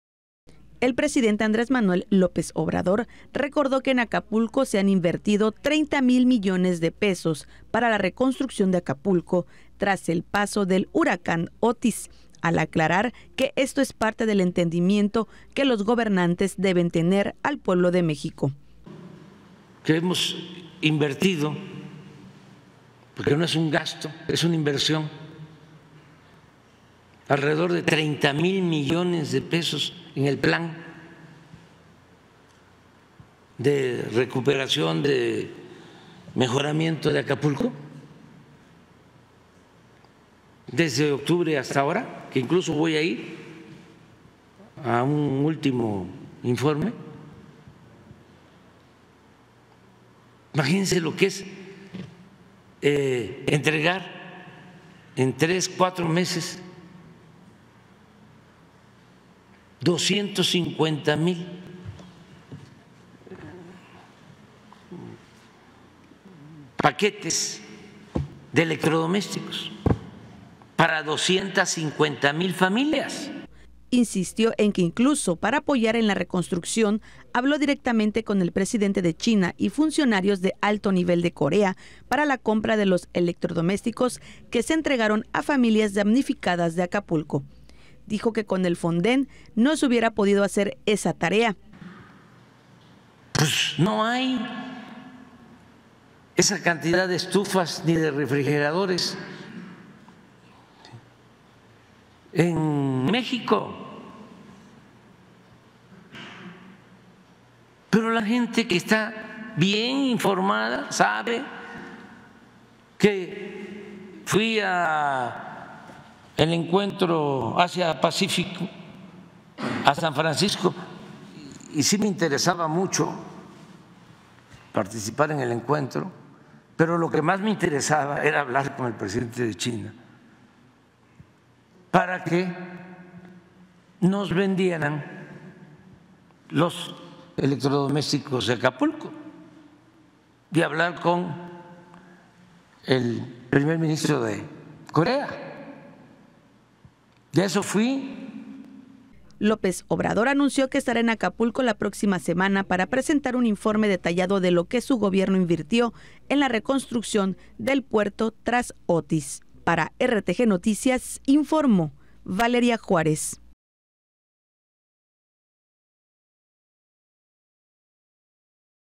RTGnoticias | 30 mil mdp invertidos en recuperación de Acapulco por Otis, destaca AMLO
El Presidente Andrés Manuel López Obrador recordó que en Acapulco se han invertido 30 mil millones de pesos para la reconstrucción de Acapulco, tras el paso del huracán Otis,  al aclarar que esto es parte del entendimiento que los gobernantes deben tener al pueblo de México.